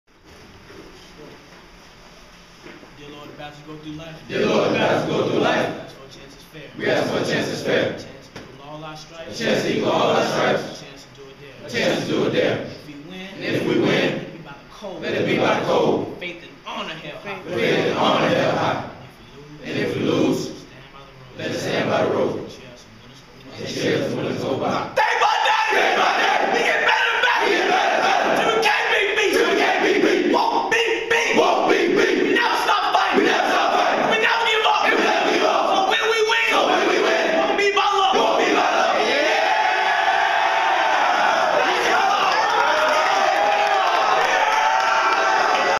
Awesome fooball players inspiring pre game prayer
Martez Anderson, Lakewood Spartan may be small but this prayer, his player ethic, and spirit inspire us all to be better players and Coaches. Lakewood H.S. of St. Petersburg Florida.